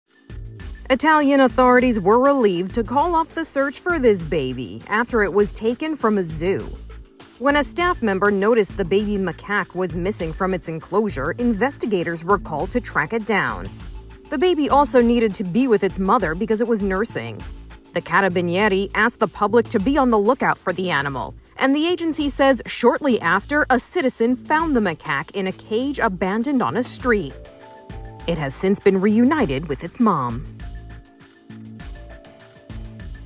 Stolen Baby Macaque Returned to Zoo in Italy
Italian authorities were relieved to call off the search for a baby macaque, after it was taken from a zoo. When a staff member noticed it was missing from its enclosure, investigators were called to track it down. The baby also needed to be with its mother because it was nursing. The Carabiniere asked the public to be on the lookout for the animal.